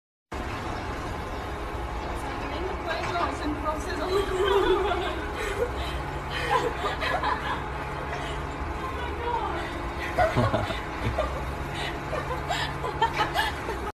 Now I've seen it all - tic tac toe using excavator-mounted hydraulic jackhammer
It takes me ENTIRE DAY to give you 15 minutes fun.